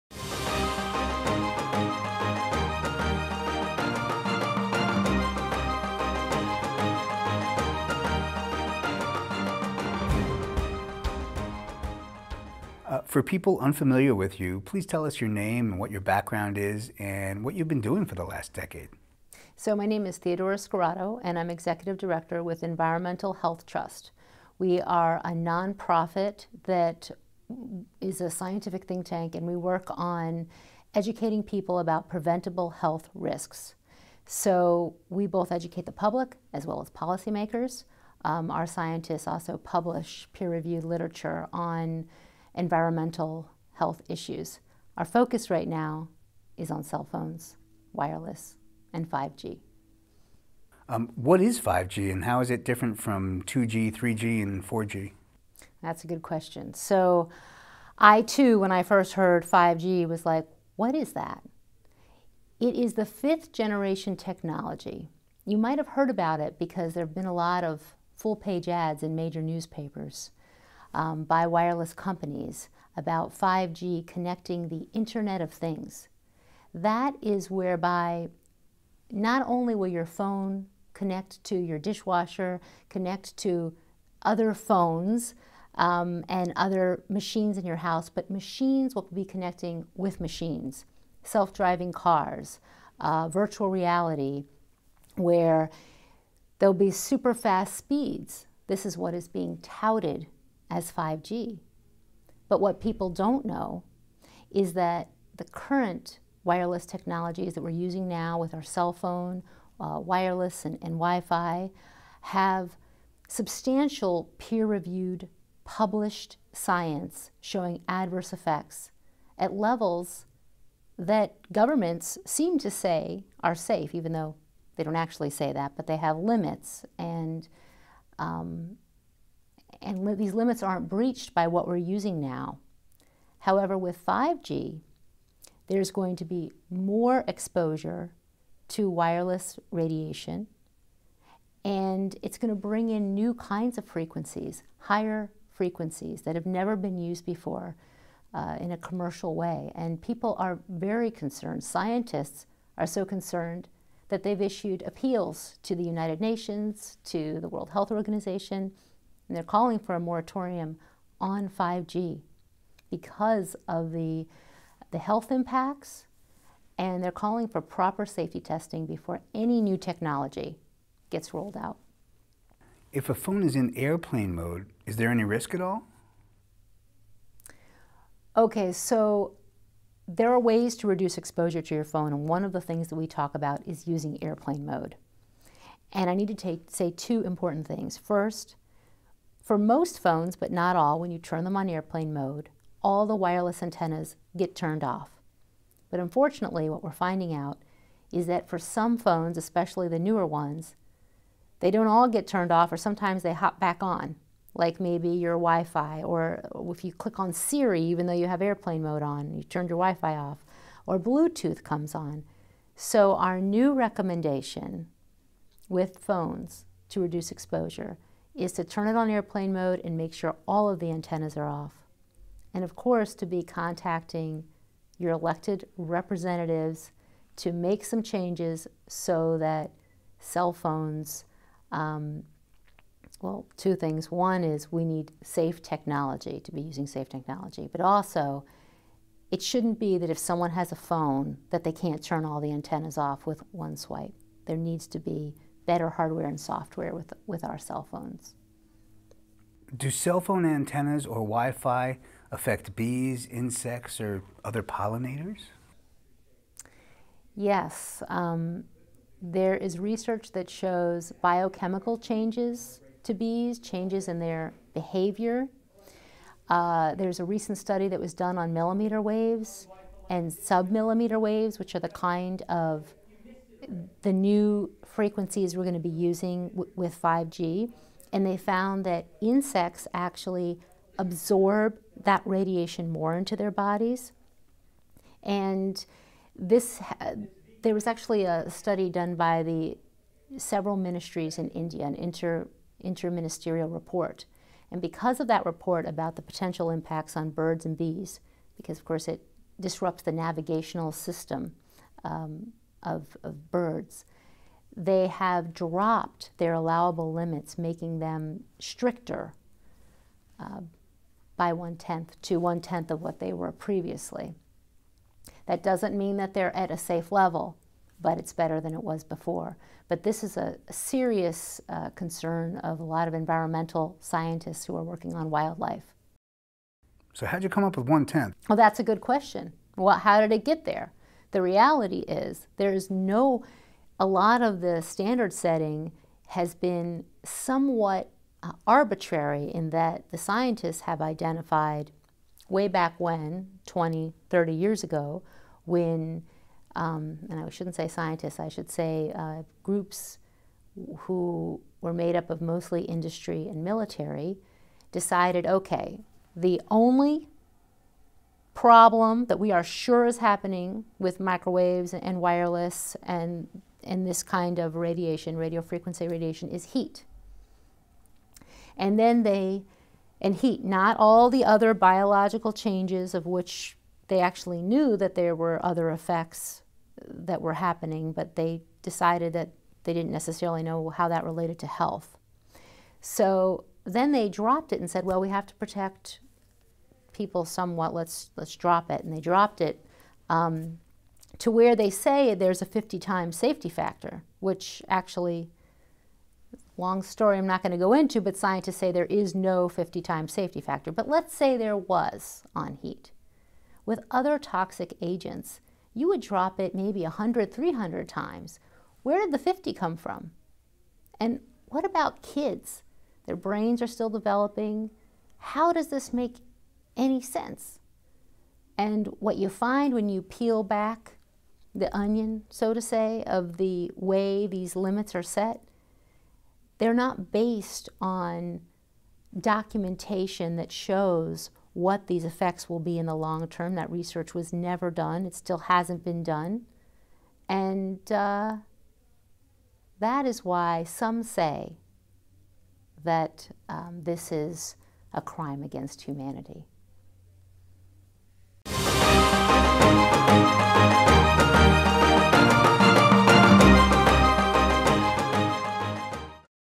What Is 5G(Fifth Generation Cellular Network Technology That Provides Broadband Access) And How Does
What Is 5G(Fifth Generation Cellular Network Technology That Provides Broadband Access) And How Does It Affect Our Health?

The Truth About Cell Phone Radiation and 5G by Theodora Scarato

Theodora Scarato directs EHT programs and coordinates scientific programs in the US and internationally with EHTs Senior Science Advisors.  Scarato is lead policy analyst and researcher for the EHT database on international actions– the most comprehensive collection of information on policy actions on cell phones and wireless. She previously worked with EHT as Director of Educational Resources and Public Affairs, developing educational resources for communities and governments.

She has co-founded several organizations both locally and nationally that address environmental health and safety concerns. As a practicing clinical psychotherapist, her two decades of work with children and adolescents includes directing an intensive  special education therapy program in Montgomery County Schools and working a psychotherapist at an ADHD clinic. Her research interests include not only the effects from radiation exposures but also the social emotional effects of technology overuse.